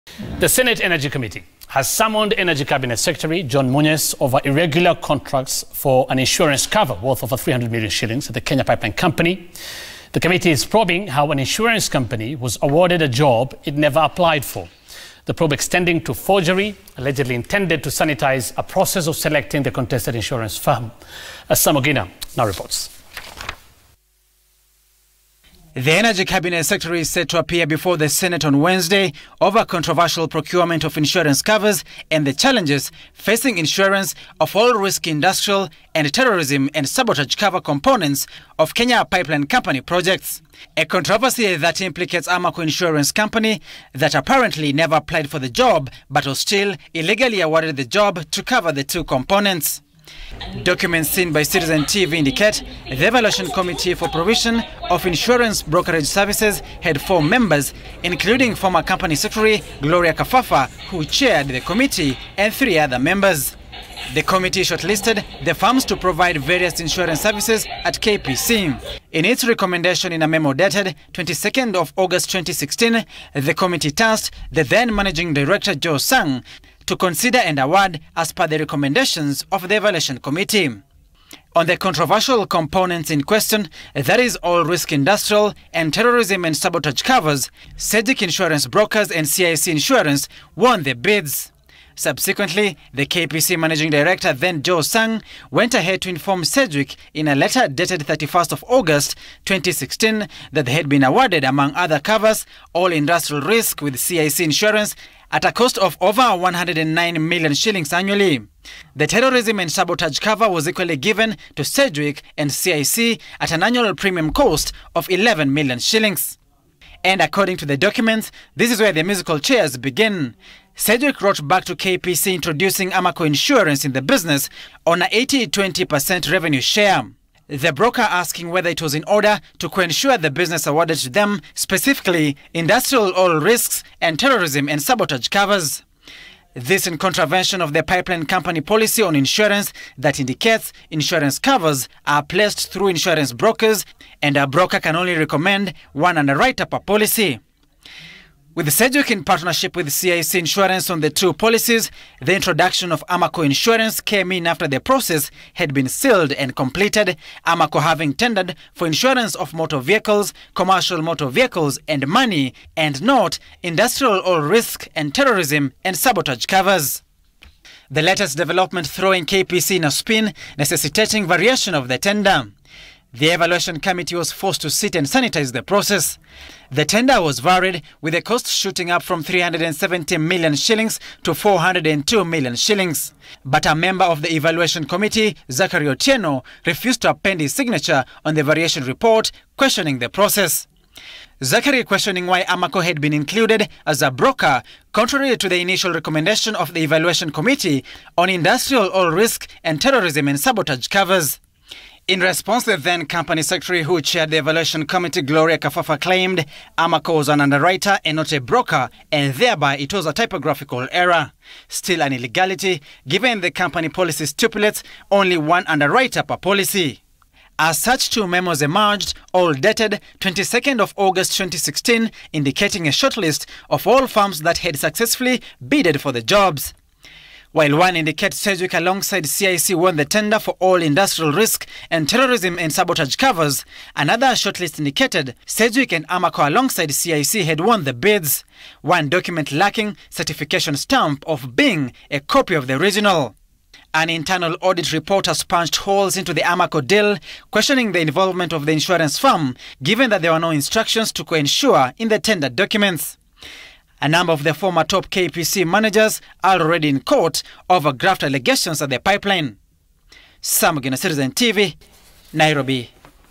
Petroleum CS Munyes summoned over insurance scandal at KPC
The senate energy committee has summoned petroleum Cabinet Secretary John Munyes over irregular contract for an insurance cover worth over 300 million shillings at the Kenya pipeline company. The committee is probing how an insurance company was awarded a job it never applied for. The probe extending into forgery, allegedly intended to sanitize a process of selecting the contested insurance firm.